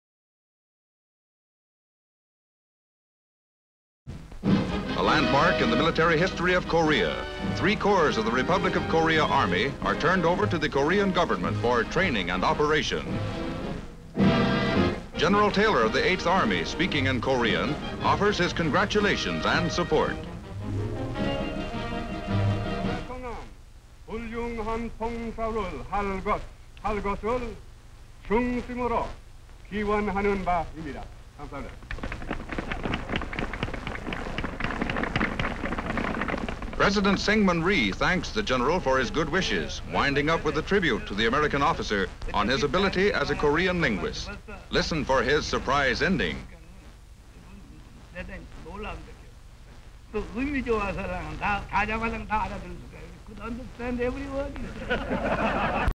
Three new Army Corps are turned over to the republic of Korea and address of cong...HD Stock Footage
Three new Army Corps are turned over to the republic of Korea and address of congratulation in Korea.

New strength for the Republic of Korea Army in Korea. Three new Army corps are turned over to the Republic of Korea for training and operation. Flag of Republic of Korea and two another flags. In the address of congratulation , US General Maxwell Taylor speaks in Korean and a surprised President Syngman Rhee turns to English in reply. Location: Korea. Date: April 29, 1954.

57,000+ broadcast-quality historic clips for immediate download.